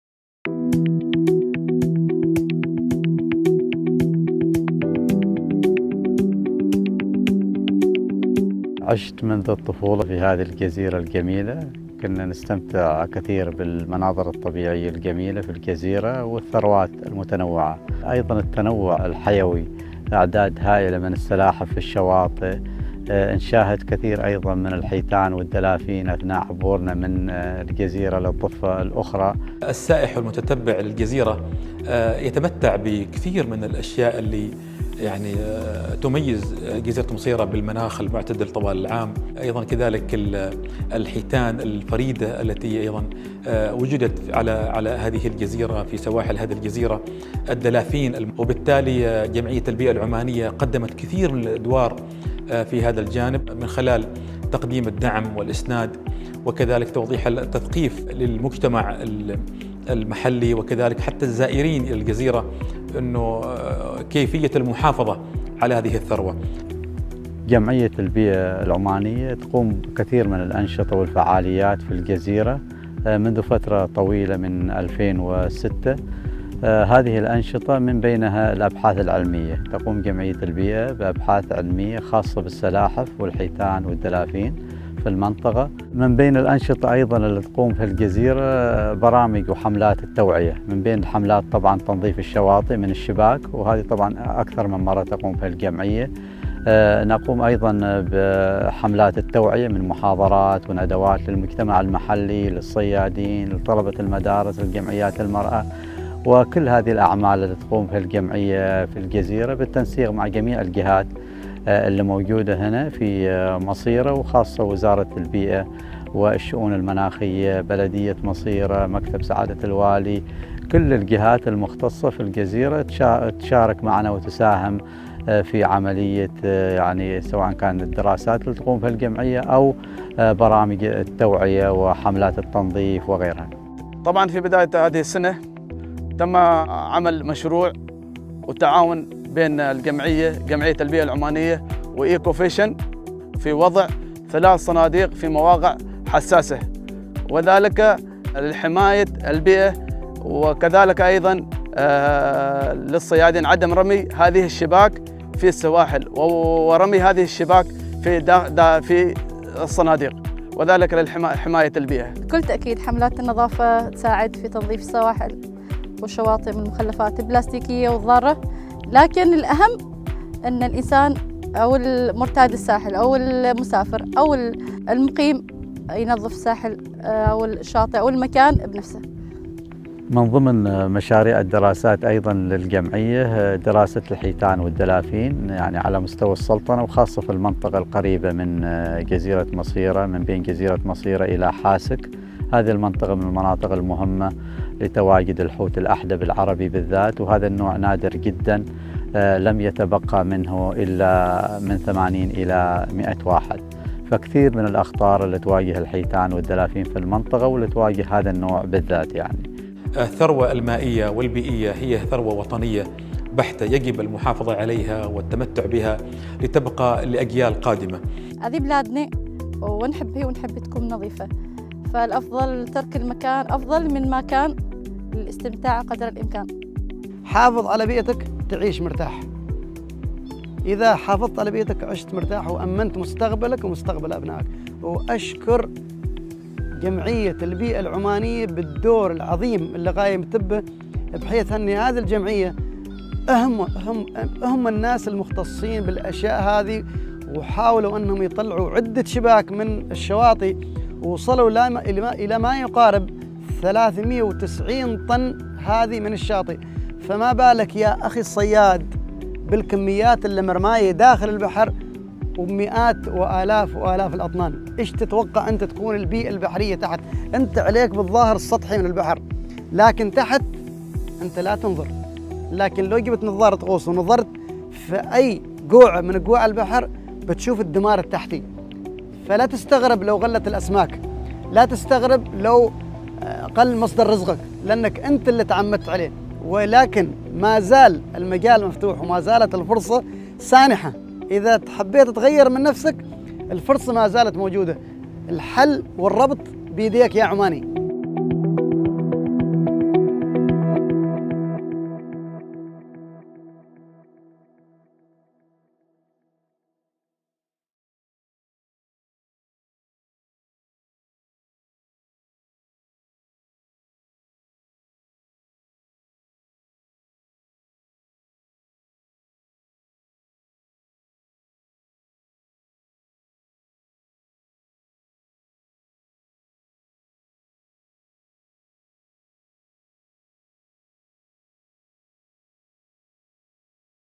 Environment Society of Oman, partners raise awareness on marine life protection
Ghost fishing causes immense damage to sea animals and harms oceans worldwide. Take a look at how Environment Society of Oman and their partner @renaissancesaog who brought together the community of Masirah to help identify ways to protect the marine life on the island.

Video credit: @boomaoman